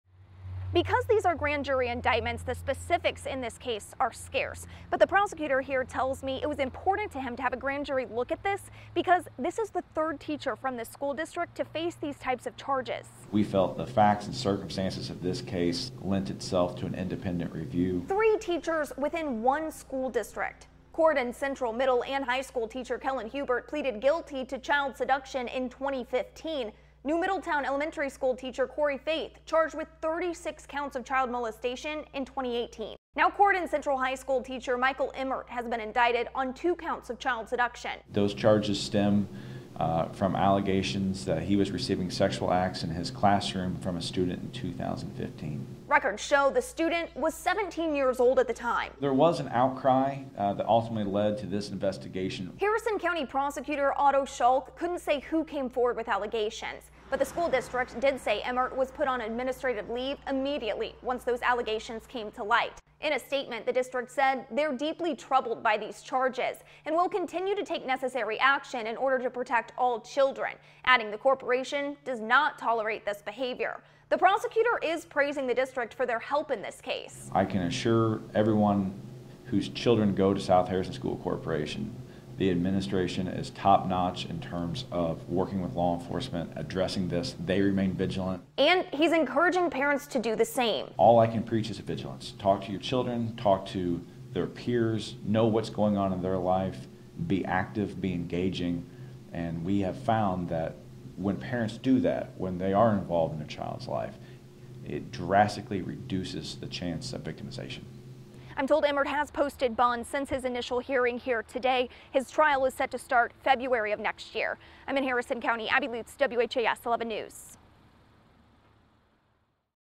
Corydon Central teacher indicted on child seduction charges
Court documents say Michael Emmert, now 42, engaged in oral intercourse with a female student who was at least 16 years old in 2015.